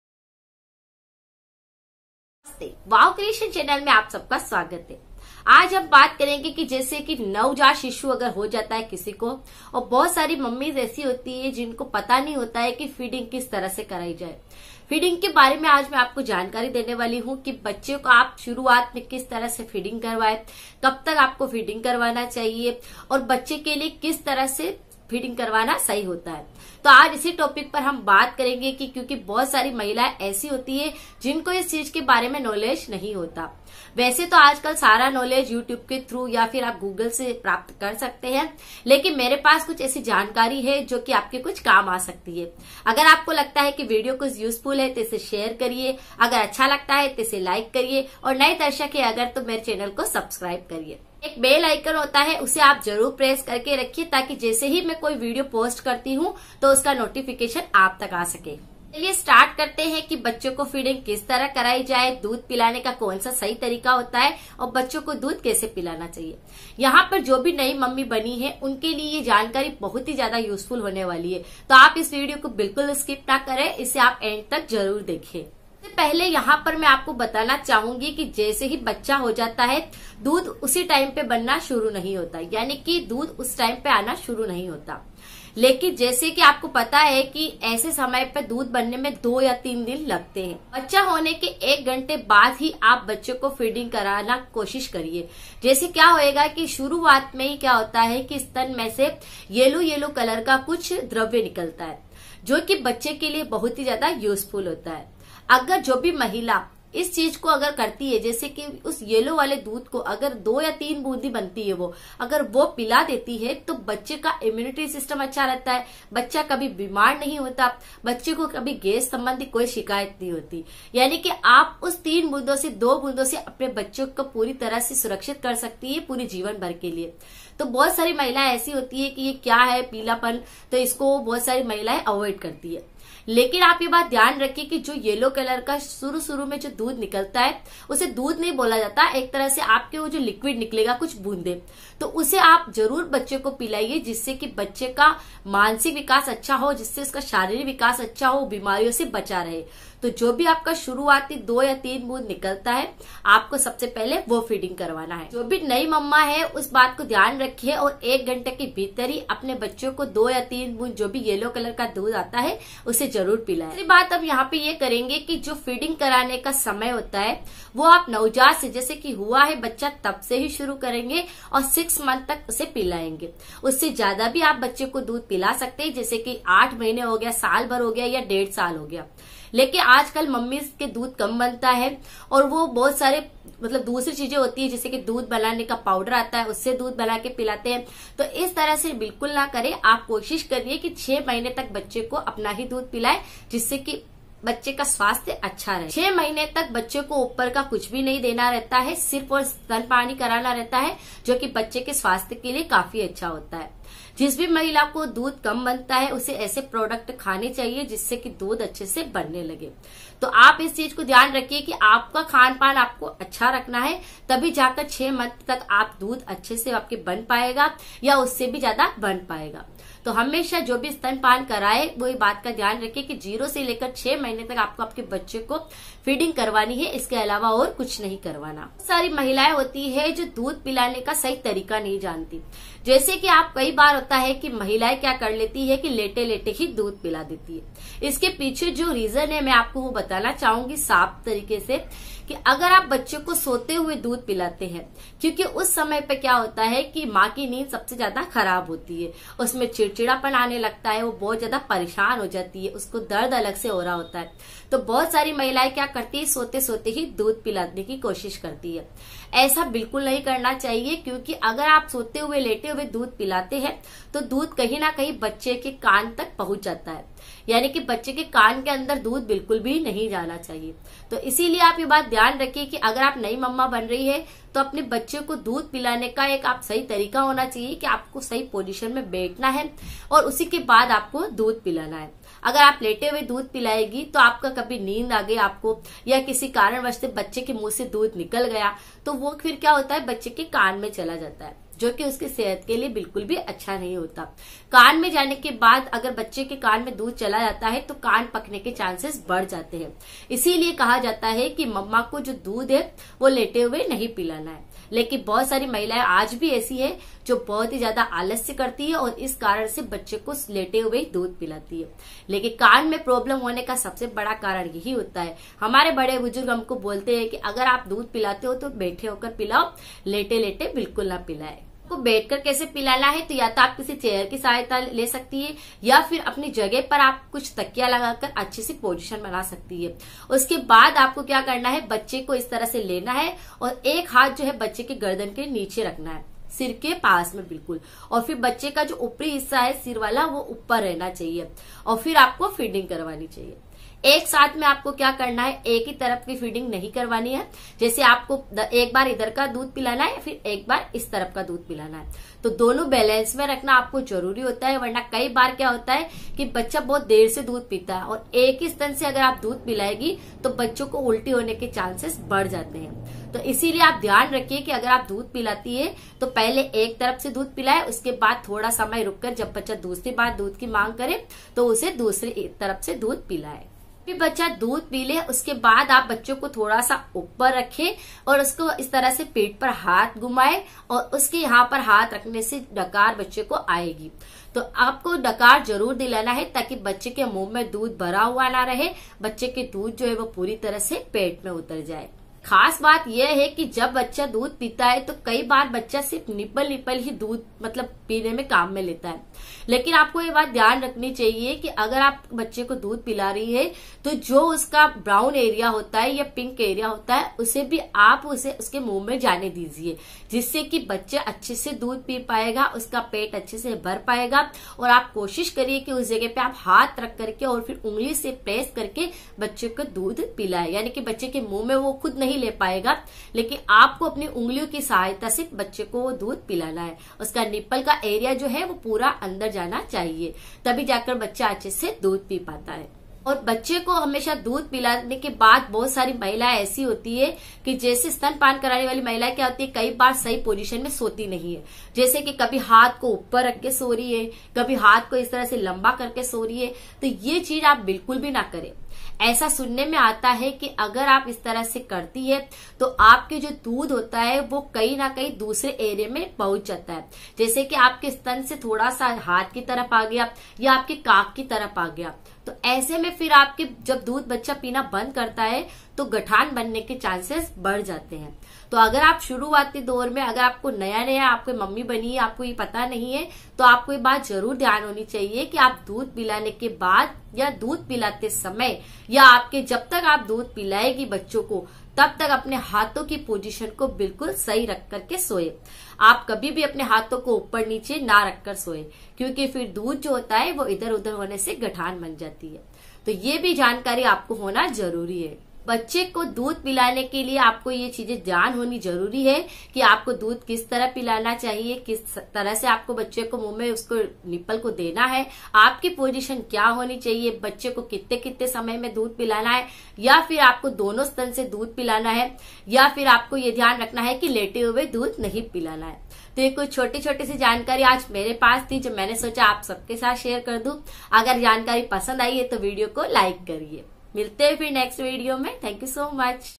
नवजात बच्चो को दूध पिलाने का सही तरीका ।नवजात को दूध पिलाने की जानकारी।Breastfeeding Idea
नवजात को कैसे दूध पिलाये यह बहुत सी महिलाए नही जानती लेकिन सही जानकारी होना बहुत जरूरी है जो भी नई माता बनी है या जिसे दूध पिलाने का तरीका नही पता तो यह वीडियो आपके लिए काफी फायदेमंद रहेगा इसलिए वीडियो में दी गयी जानकारी के हिसाब से अगर आप दूध पिलाते है या बच्चो का स्वास्थ्य अच्छा रहेगा और कोई भी नुकसान नही होगा।